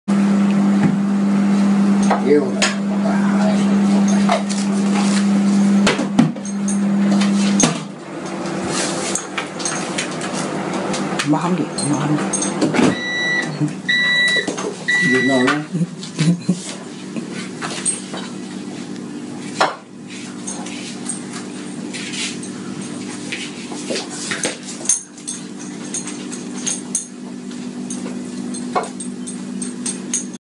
Mabel the Golden: Golden Retriever puppy chasing tale and getting dizzy (13 weeks)
Mabel chasing her tale and then getting dizzy! Apologies for the poor video quality, these videos were taken on iPhones and the videos I will do from now on will be on my camera - so hopefully much better quality!